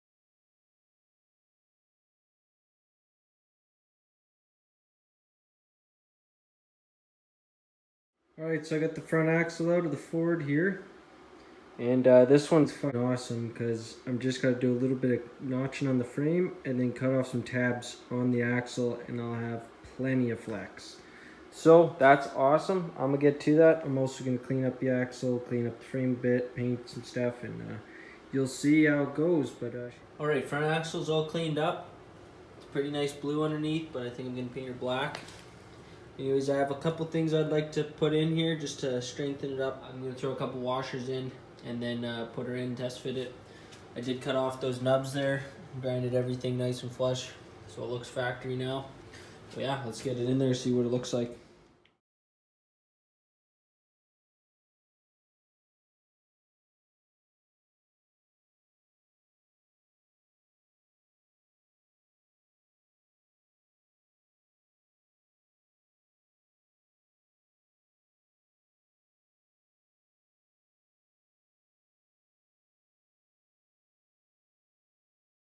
Lawnmower front axle mods,tips
Your front axle is a very important part of your off-road mower! Building a strong reliable swing/I beam is key to success!

These cast iron I beam front axles are awesome axles to start with as they are heavy duty from factory and don’t need a ton of mods to work great in your build!

I also use a lot of other style front axles and you can seem me build them in my other YouTube build series videos!

Our Instagram has lots of pictures and videos of our setups in action!
Keep on mowin